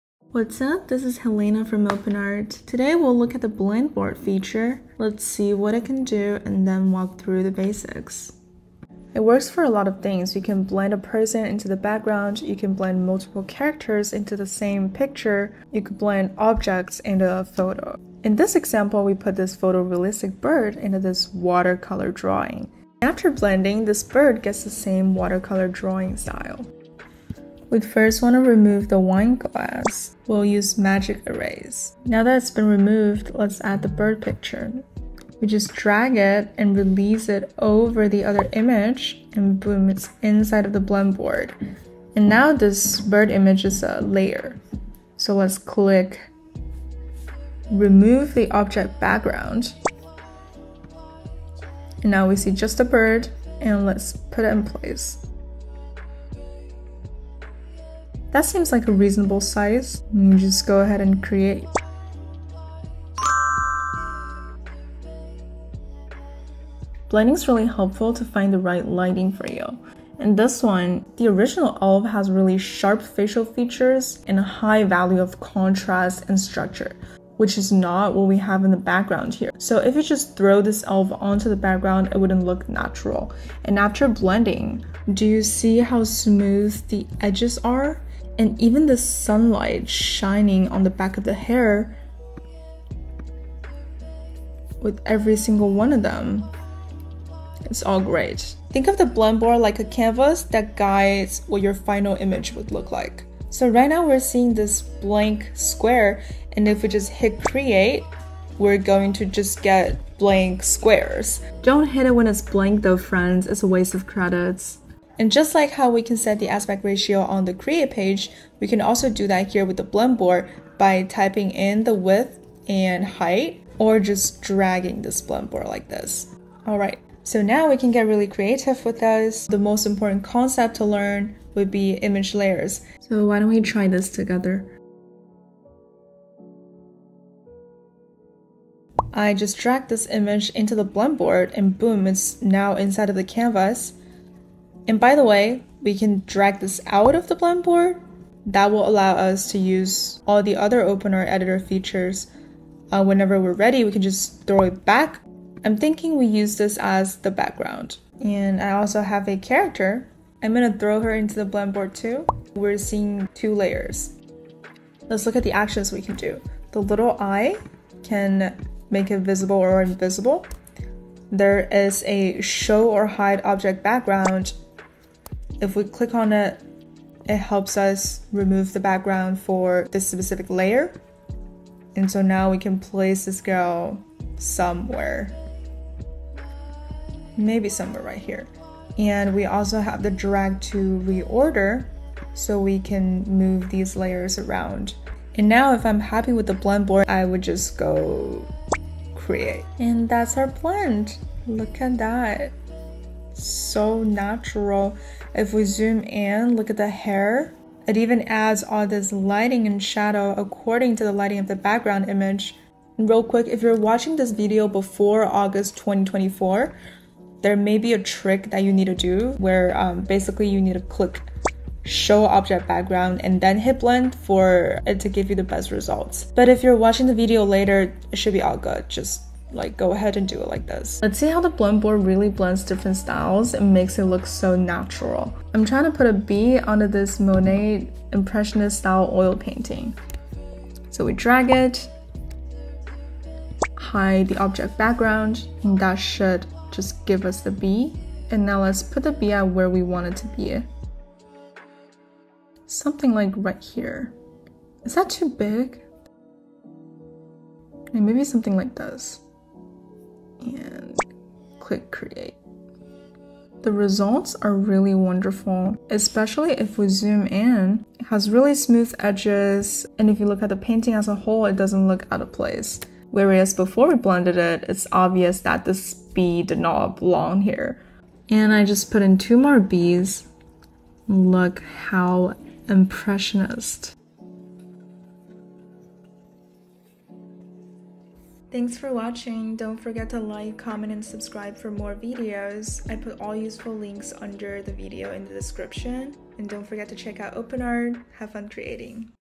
Combine Characters with AI Blend (Beta Feature Demo - OpenArt AI)
Elevate your creativity with OpenArt's Blend feature, an advanced AI tool that allows you to seamlessly combine elements within your images. Whether you're looking to place multiple characters in one scene, add objects to a background, or apply patterns to clothing, OpenArt makes it easy and intuitive.

🧭 Nav
0:00 Blend Board
0:23 Example 1 - watercolor
1:07 Example 2 - Elf
2:15 Example 3 - background
3:51 Example 4 - oilpainting

✨ Why Use OpenArt Blend?

🎨 Combine multiple characters into a single scene
🏞️ Seamlessly integrate objects with any background
👕 Apply patterns and designs to clothing effortlessly
🖥️ User-friendly interface for quick and easy edits
🌟 High-quality results with advanced AI technology
⏱️ Save time and enhance your creative projects

In this tutorial, we'll guide you through the process of using OpenArt Blend to create stunning composite images. Perfect for digital artists, content creators, and anyone looking to enhance their visual projects with AI, this feature offers endless possibilities.

Follow our step-by-step guide to learn how to make the most of OpenArt's Blend feature. Discover how simple it is to create professional-looking composites without the need for expensive software or extensive editing skills.

Unlock the power of OpenArt Blend and start combining elements in your images with ease today!